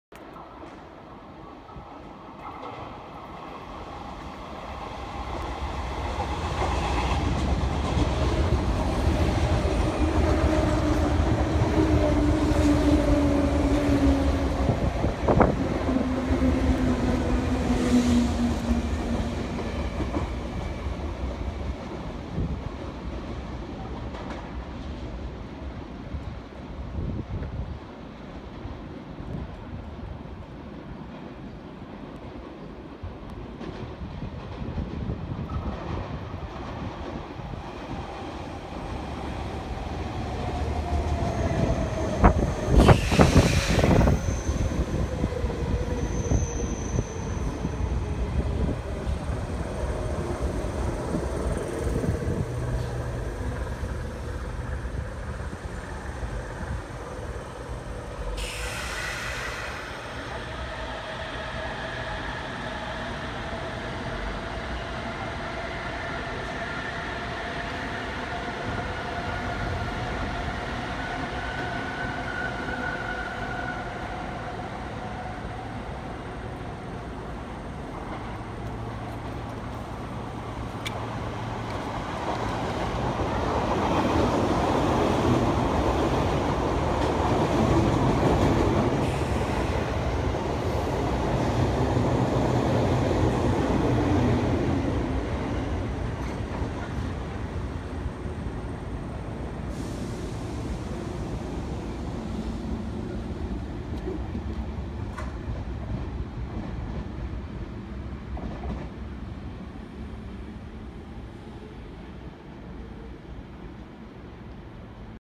相鉄線 平沼橋駅 9000系 10000系 通過 7000系 発着
2017年6月3日iPhone6sで撮影